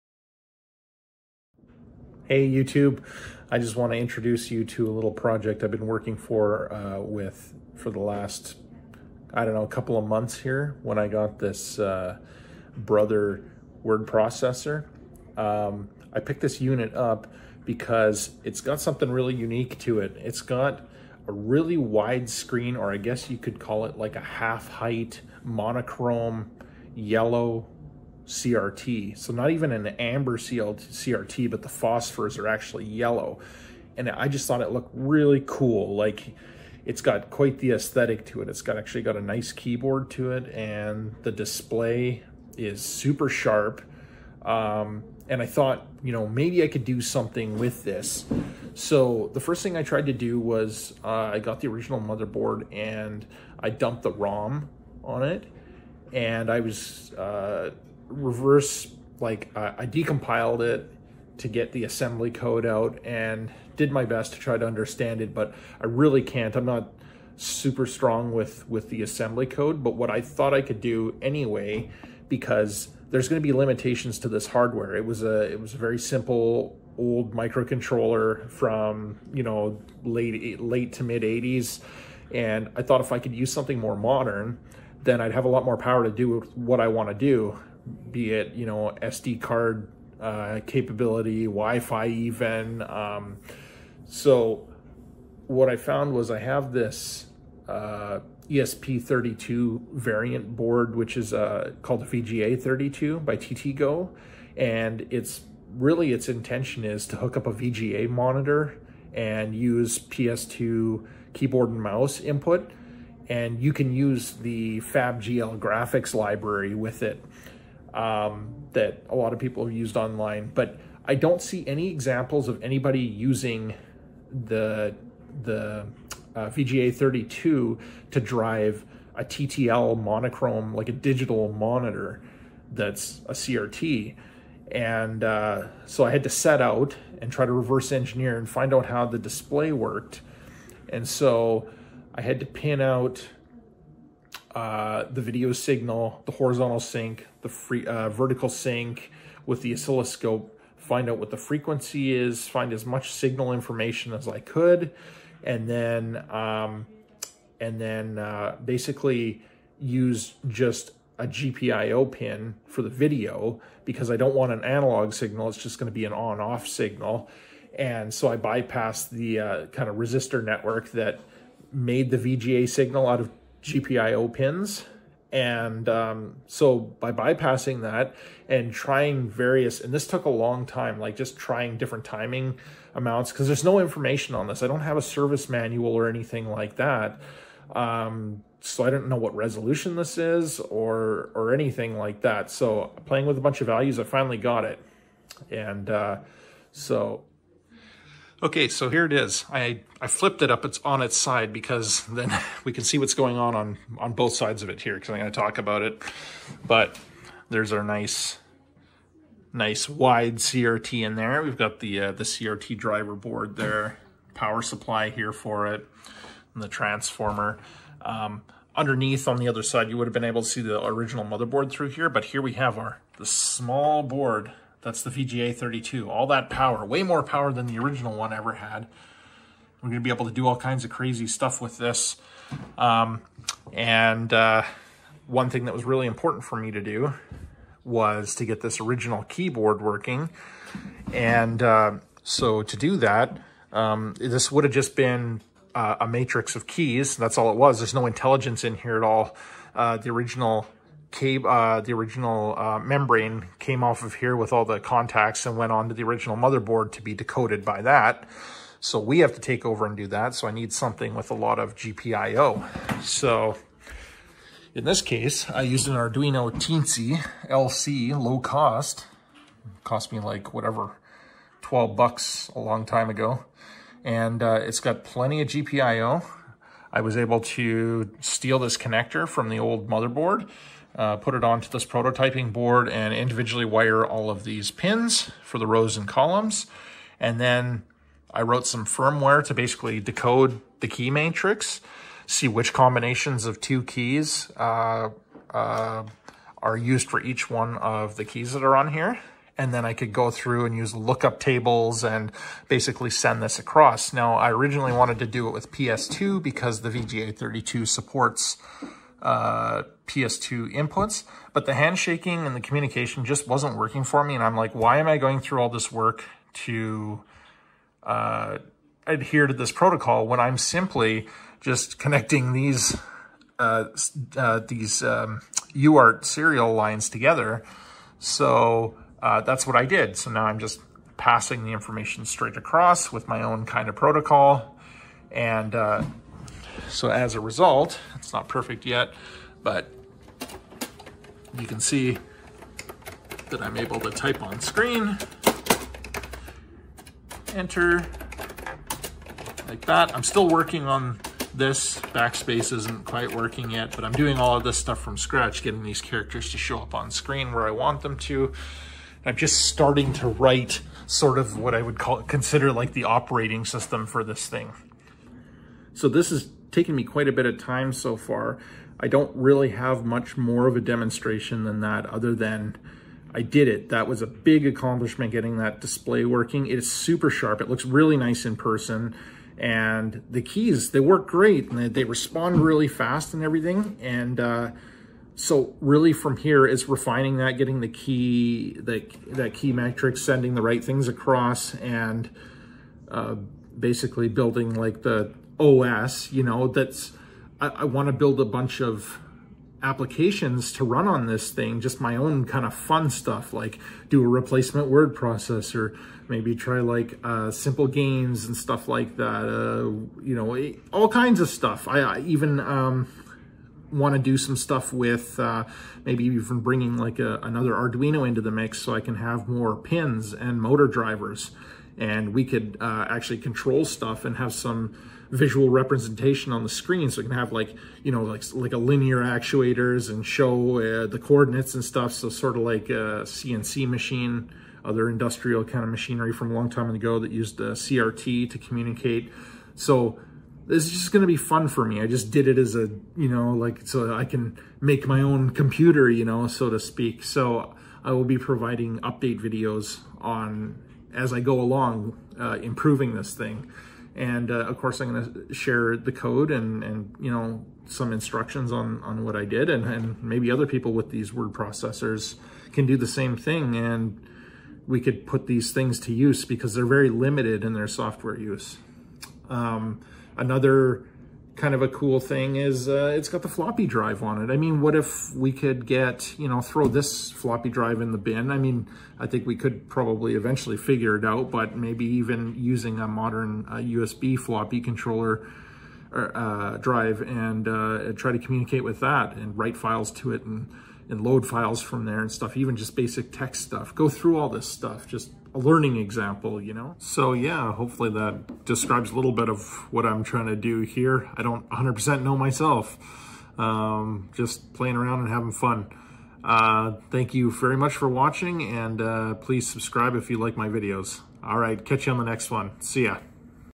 I hacked a Typewriter to run my own code! Brother Word Processor
I reverse engineered a Brother word processor, which is essentially a smart typwriter that lets you edit before you print. I outline what I had to do to get it to the point where I can write my own software for it.